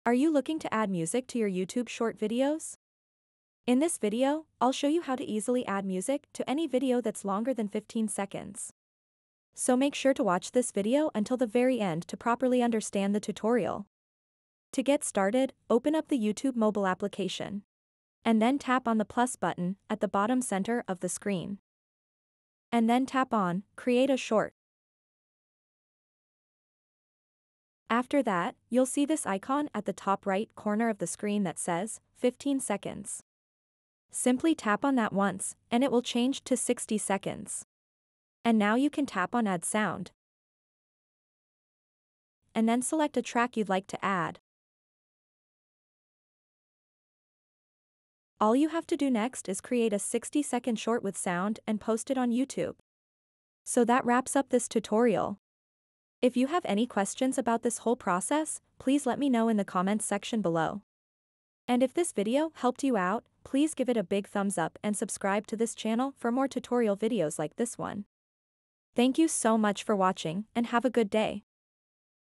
How To Add Music In Shorts More Than 15 Sec
Are you ready to make your YouTube Shorts longer than just 15 seconds? Do you want to add music but don't know how? Look no further! This video is the perfect guide for anyone wanting to learn how to add music in Shorts that are longer than 15 seconds. We'll walk you through the entire process step-by-step, so by the end of it, adding music to your shorts will be a breeze. Don't miss out - watch our video now and learn how to add music in shorts more than 15 seconds!